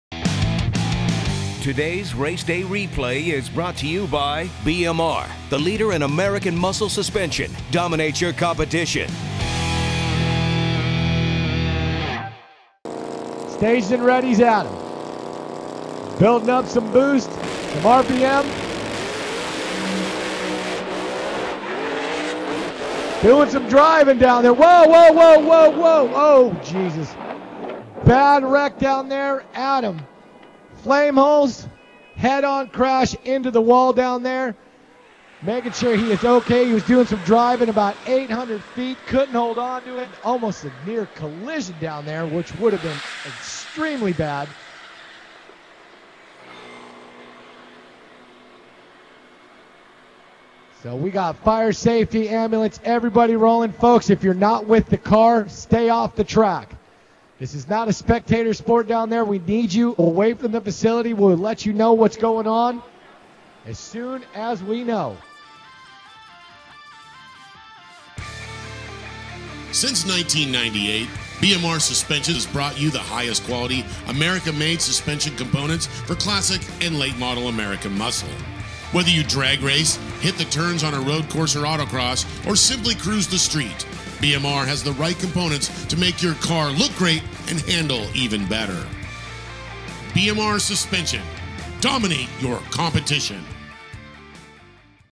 Adam Flamholc Huge Crash At The NMRA NMCA Super Bowl
Adam Flamholc had a terrifying crash at the NMRA/NMCA Super Bowl during round one of Xtreme Pro Mod. His blown Corvette crossed the center line at the top end and made hard contact with the wall. Thankfully, Flamholc wasn't injured and will be ready to race again soon.